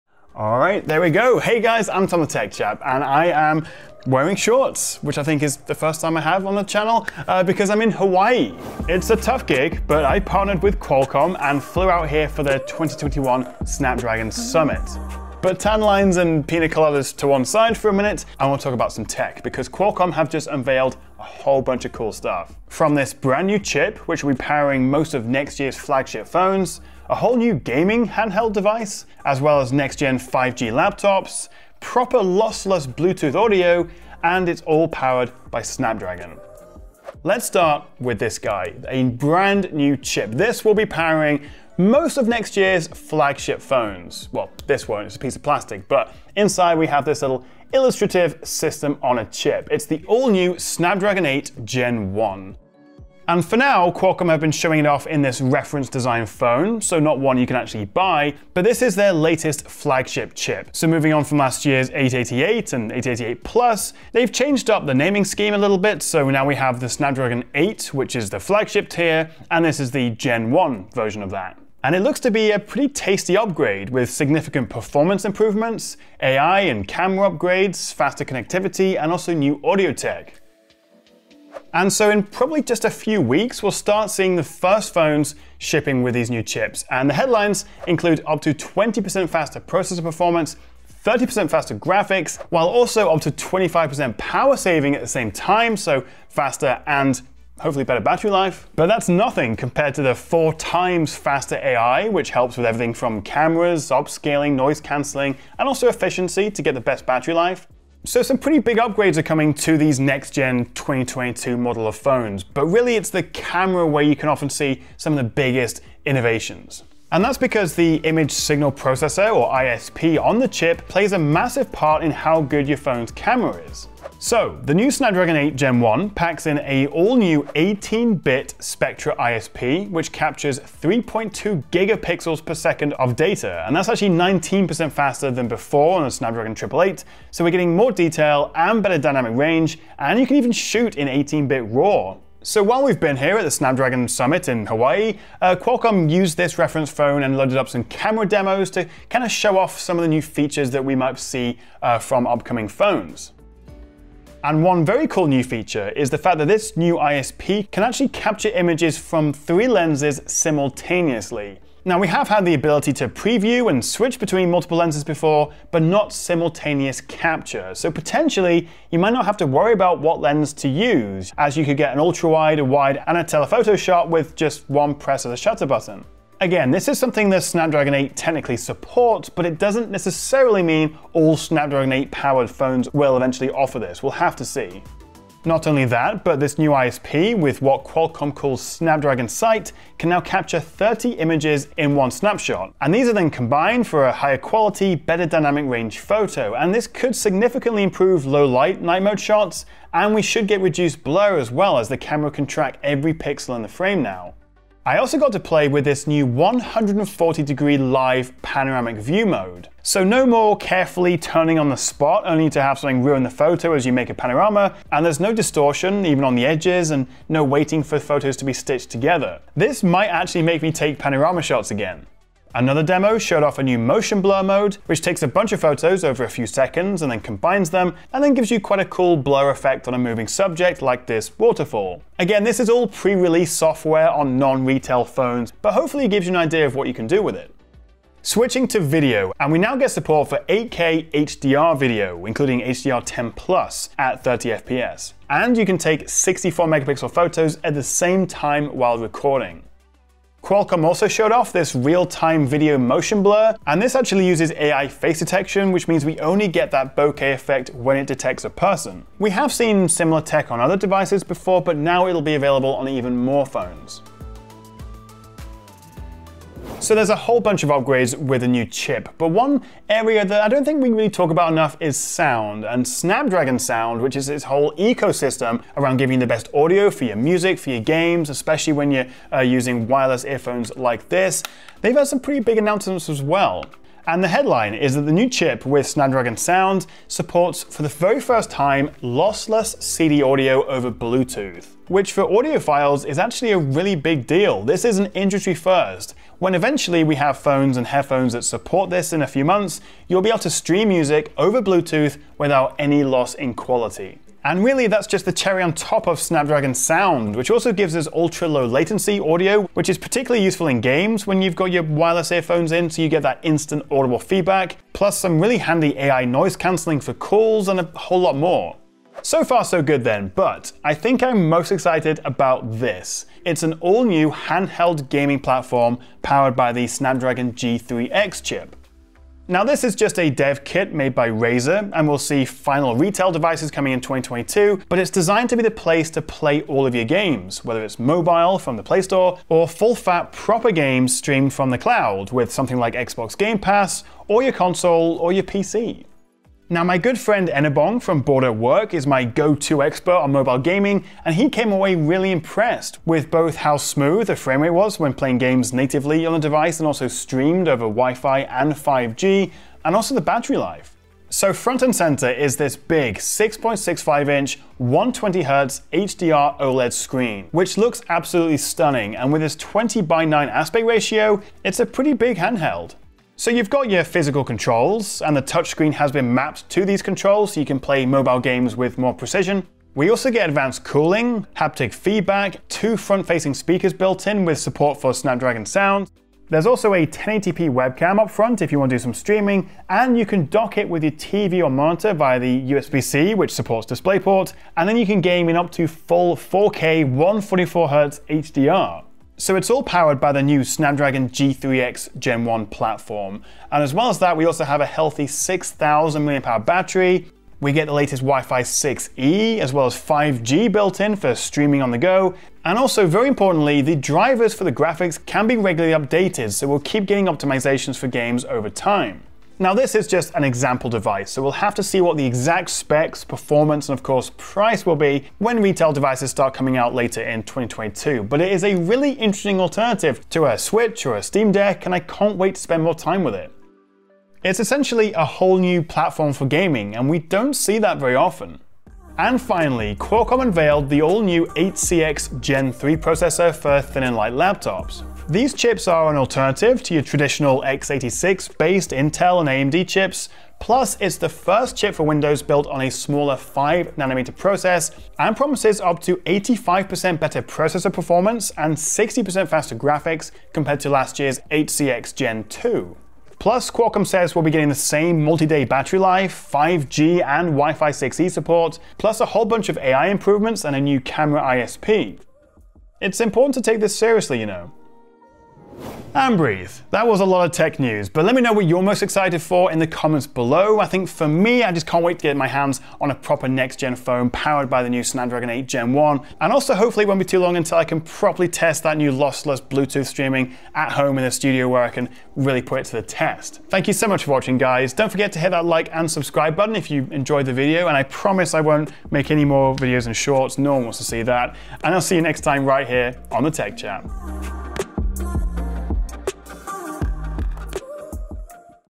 Snapdragon 8 Gen 1 Impressions! (Benchmark, Camera & Gaming)
From Qualcomm Snapdragon Summit 2021 - I'm hands-on with the flagship Snapdragon 8 Gen 1 chip, new handheld gaming device, Snapdragon Sound, and next-gen 5G laptops!

00:00 Snapdragon Summit! (in SHORTS)
00:50 SD 8 Gen 1 - Specs & Camera
04:26 Snapdragon SOUND!
05:19 Next Gen Handheld Gaming
07:20 5G Laptops!
07:56 Most Exciting Tech?